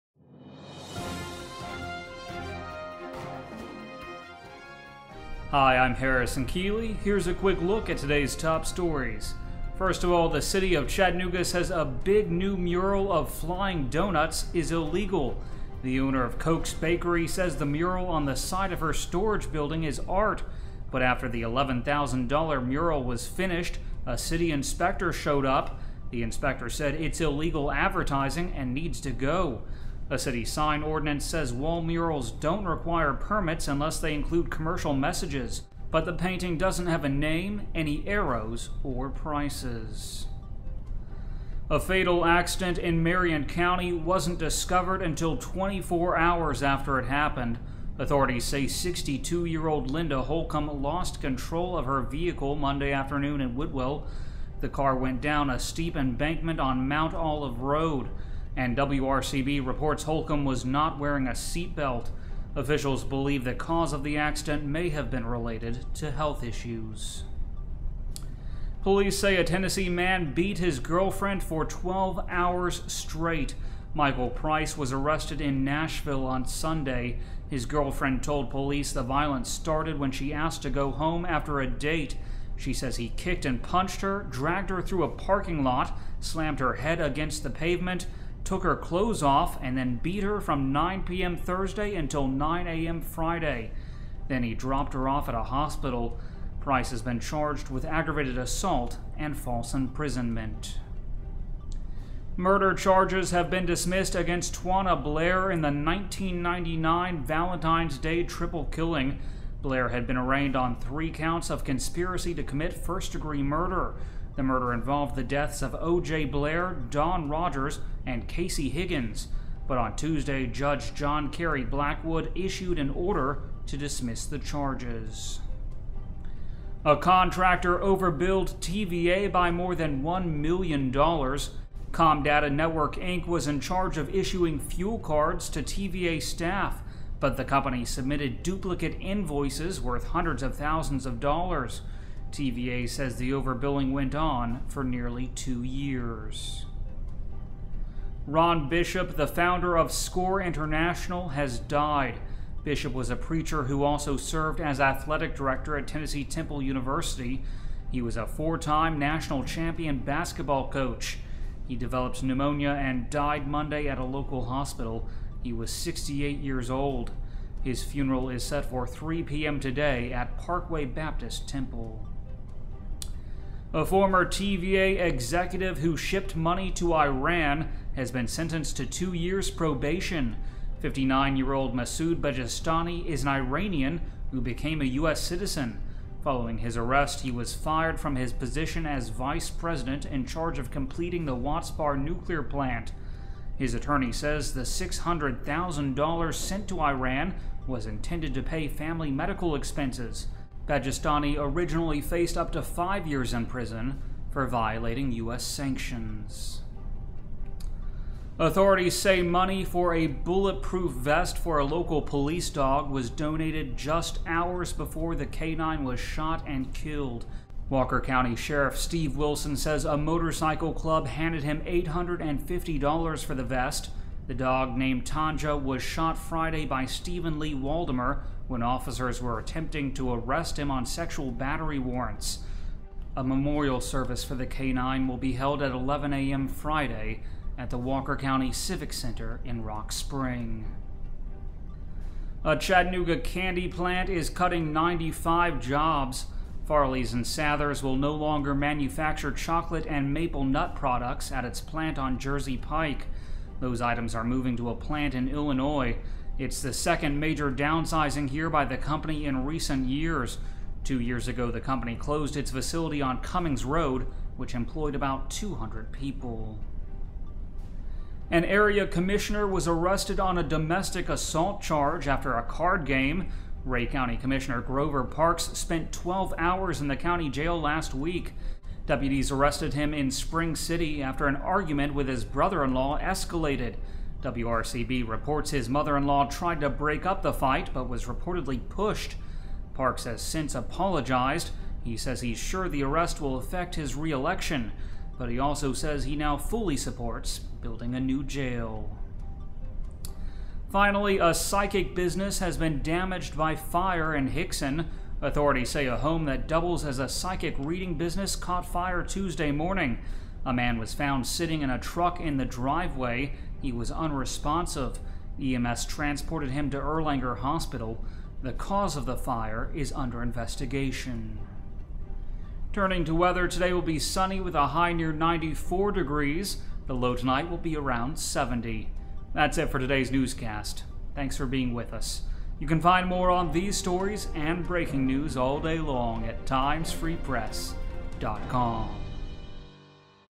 Chattanooga Update - June 18, 2014
The City of Chattanooga says a big new mural of flying doughnuts is illegal.

The owner of Koch's Bakery says the mural on the side of her storage building is art.

But after the $11,000 dollar mural was finished, a city inspector showed up.

The inspector said it's illegal advertising and needs to go.

A city sign ordinance says wall murals don't require permits unless they include commercial messages.

But the painting doesn't have a name, any arrows - or prices.

A fatal accident in Marion County wasn't discovered until 24 hours after it happened.

Authorities say 62-year-old Linda Holcomb lost control of her vehicle Monday afternoon in Whitwell.

The car went down a steep embankment on Mount Olive Road.

And WRCB reports Holcomb was not wearing a seat belt.

Officials believe the cause of the accident may have been related - to health issues.
Police say a Tennessee man beat his girlfriend for 12 hours straight.

Michael Price was arrested in Nashville on Sunday.

His girlfriend told police the violence started when she asked to go home after a date.

She says he kicked and punched her, dragged her through a parking lot, slammed her head against the pavement, took her clothes off, and beat her from 9 p.m. Thursday until 9 a.m. Friday.

Then he dropped her off at a hospital.

Price has been charged with aggravated assault - and false imprisonment.
Murder charges have been dismissed against Twanna Blair in the 1999 Valentine's Day triple killing.

Blair had been arraigned on three counts of conspiracy to commit first-degree murder.

The murder involved the deaths of O.J. Blair, Dawn Rogers and Cayci Higgins.
But on Tuesday, Judge Jon Kerry Blackwood issued an order - to dismiss the charges.
A contractor overbilled TVA by more than $1 million dollars.

Comdata Network Inc. was in charge of issuing fuel cards to TVA staff.

But the company submitted duplicate invoices worth hundreds of thousands of dollars.

TVA says the overbilling went on - for nearly two years.

Ron Bishop, the founder of SCORE international, has died.

Bishop was a preacher who also served as athletic director at Tennessee Temple University. He was a four-time national champion basketball coach.

He developed pneumonia and died Monday at a local hospital. He was 68 years old.

His funeral is set for 3 p.m. today at Parkway Baptist Temple.
A former TVA executive who shipped money to Iran has been sentenced to two years probation.

59-year-old Masoud Bajestani is an Iranian who became a U.S. citizen. Following his arrest, he was fired from his position as vice president in charge of completing the Watts Bar Nuclear plant.

His attorney says the $600,000 dollars sent to Iran was intended to pay family medical expenses.

Bajestani originally faced up to five years in prison - for violating U.S. sanctions.

Authorities say money for a bulletproof vest for a local police dog was donated just hours before that K-9 was shot and killed.

Walker County Sheriff Steve Wilson says a motorcycle club handed him $850 dollars for the vest.

The dog, named Tanja, was shot Friday by Steven Lee Waldemer, when officers were attempting to arrest him on sexual battery warrants.

A memorial service for the K-9 will be held at 11 a.m. Friday at the Walker County Civic Center in Rock Spring.

A Chattanooga candy plant is cutting 95 jobs.

Farley's & Sathers will no longer manufacture chocolate and maple nut products at its plant on Jersey Pike.

Those items are moving to plants in Illinois.

It's the second major downsizing here by the company in recent years.

Two years ago the company closed its facility on Cummings Road, which employed about 200 people.
An area commissioner was arrested on a domestic assault charge after a card game.

Rhea County Commissioner Grover Parks spent 12 hours in the county jail last week.

Deputies arrested him in Spring City after an argument with his brother-in-law escalated.

WRCB reports his mother-in-law tried to break up the fight but was reportedly pushed.

Parks has since apologized. He says he's sure the arrest will affect his re-election.

But he also says he now fully supports building a new jail.

Finally, a psychic business has been damaged by fire in Hixson.

Authorities say a home that doubles as a psychic reading business caught fire Tuesday morning.

A man was found sitting in a truck in the driveway. He was unresponsive.

EMS transported him to Erlanger hospital.

The cause of the fire is under investigation.